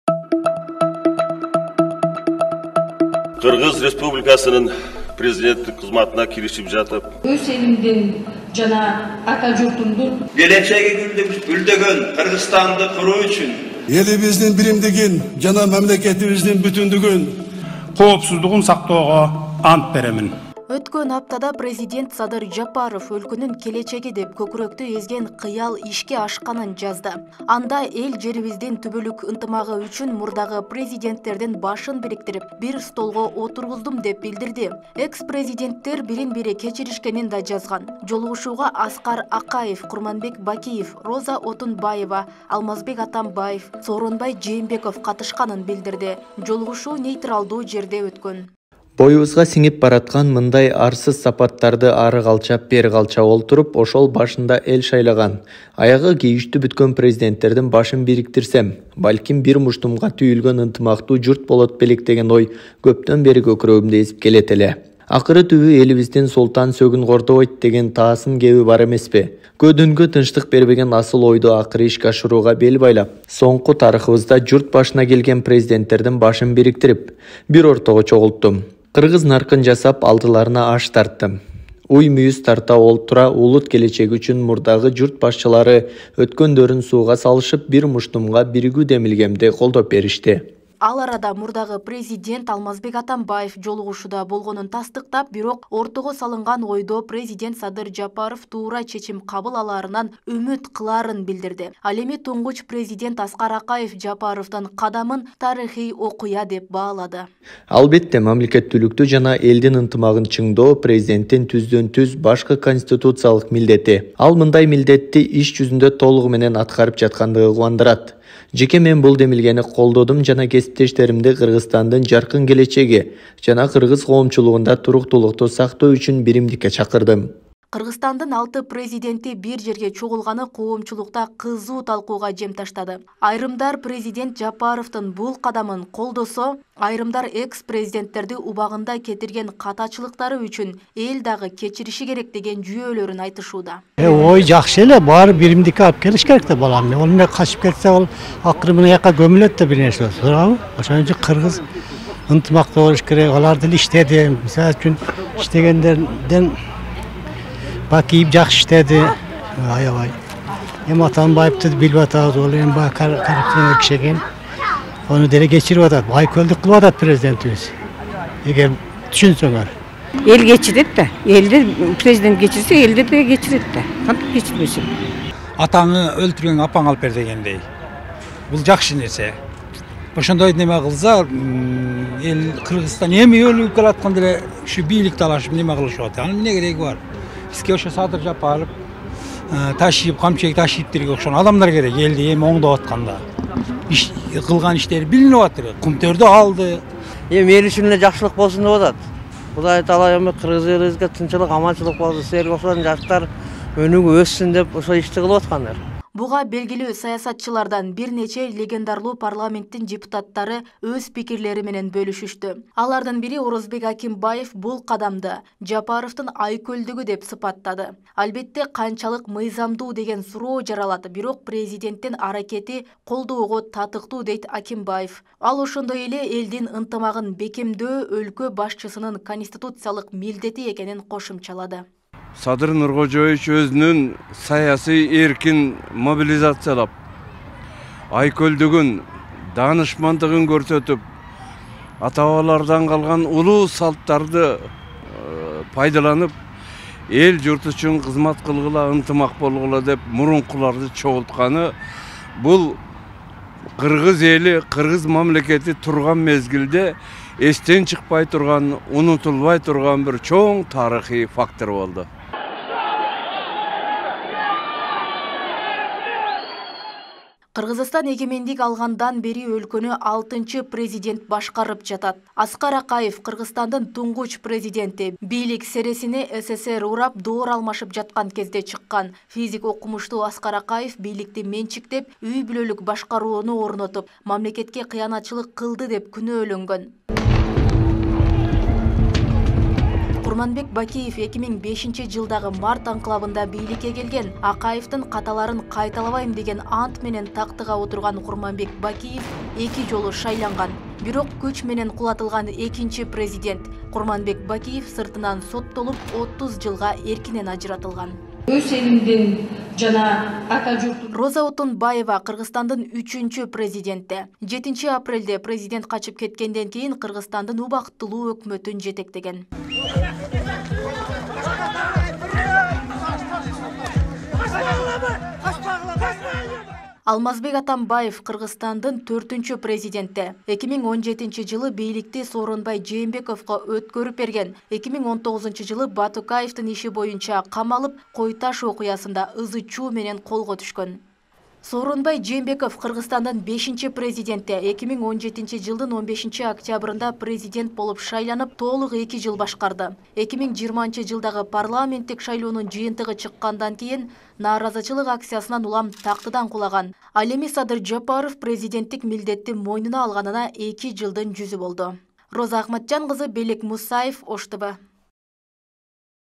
Кыргызстандын мурдагы президенттеринин башын бириктирип, бир столго олтургузганын билдирди
Президент Садыр Жапаров Кыргызстандын мурдагы президенттеринин башын бириктирип, бир столго олтургузганын билдирди